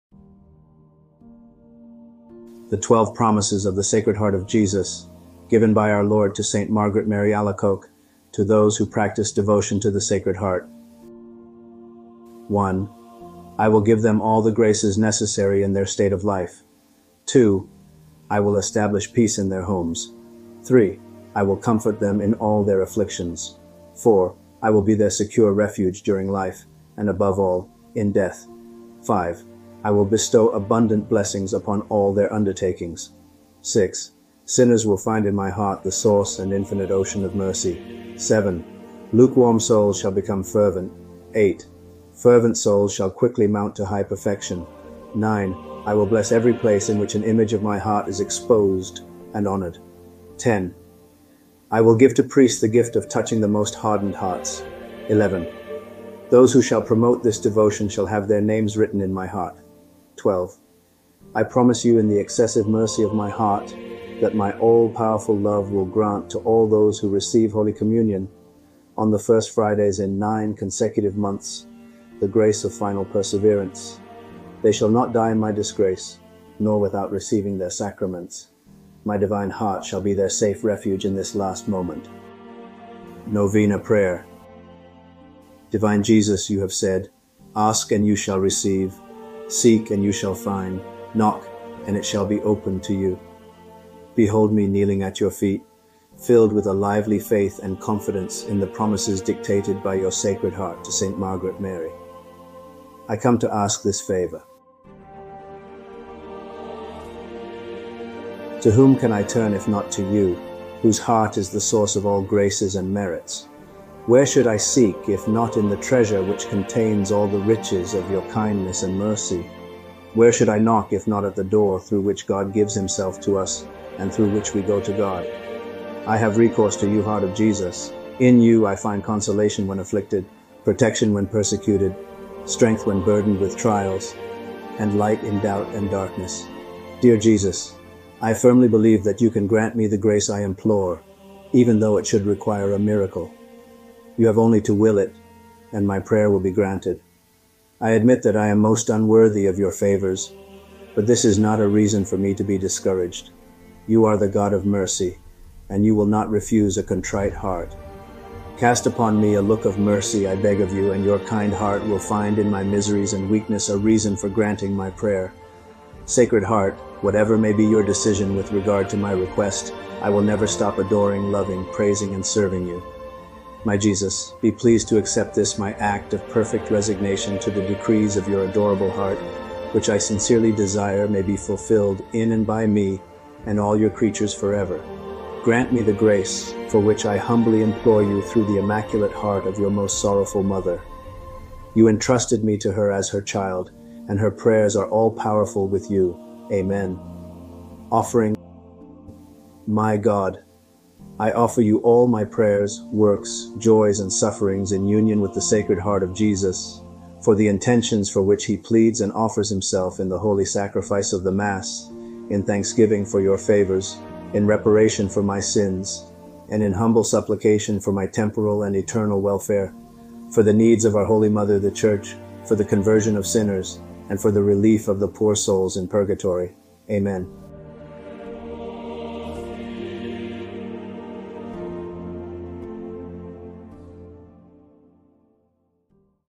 The 12 Promises of the Sacred Heart of Jesus Given to St. Margaret Mary Alacoque
1. "I will give them all the graces necessary in their state of life.
2. I will establish peace in their homes.
3. I will comfort them in all their afflictions.
4. I will be their secure refuge during life, and above all, in death.
5. I will bestow abundant blessings upon all their undertakings.
6. Sinners will find in my Heart the source and infinite ocean of mercy.
7. Lukewarm souls shall become fervent.
8. Fervent souls shall quickly mount to high perfection.
9. I will bless every place in which an image of my Heart is exposed and honored.
10. I will give to priests the gift of touching the most hardened hearts.
11. Those who shall promote this devotion shall have their names written in my Heart.
12. I promise you in the excessive mercy of my Heart that my all-powerful love will grant to all those who receive Holy Communion on the First Fridays in nine consecutive months the grace of final perseverance; they shall not die in my disgrace, nor without receiving their sacraments. My divine Heart shall be their safe refuge in this last moment."

Novena Prayer

Divine Jesus, You have said, 'Ask and you shall receive; seek and you shall find; knock and it shall be opened to you.' Behold me kneeling at Your feet, filled with a lively faith and confidence in the promises dictated by Your Sacred Heart to Saint Margaret Mary. I come to ask this favor: Mention your request).

To whom can I turn if not to You, Whose Heart is the source of all graces and merits? Where should I seek if not in the treasure which contains all the riches of Your kindness and mercy? Where should I knock if not at the door through which God gives Himself to us and through which we go to God? I have recourse to You, Heart of Jesus. In You I find consolation when afflicted, protection when persecuted, strength when burdened with trials, and light in doubt and darkness.

Dear Jesus, I firmly believe that You can grant me the grace I implore, even though it should require a miracle. You have only to will it and my prayer will be granted. I admit that I am most unworthy of Your favors, but this is not a reason for me to be discouraged. You are the God of mercy, and You will not refuse a contrite heart. Cast upon me a look of mercy, I beg of You, and Your kind Heart will find in my miseries and weakness a reason for granting my prayer.

Sacred Heart, whatever may be Your decision with regard to my request, I will never stop adoring, loving, praising, and serving You. My Jesus, be pleased to accept this my act of perfect resignation to the decrees of Your adorable Heart, which I sincerely desire may be fulfilled in and by me and all Your creatures forever.

Grant me the grace for which I humbly implore You through the Immaculate Heart of Your most sorrowful Mother. You entrusted me to her as her child, and her prayers are all-powerful with You. Amen.

Offering...

My God, I offer You all my prayers, works, joys, and sufferings in union with the Sacred Heart of Jesus, for the intentions for which He pleads and offers Himself in the Holy Sacrifice of the Mass, in thanksgiving for Your favors, in reparation for my sins, and in humble supplication for my temporal and eternal welfare, for the needs of our holy Mother the Church, for the conversion of sinners, and for the relief of the poor souls in purgatory. Amen.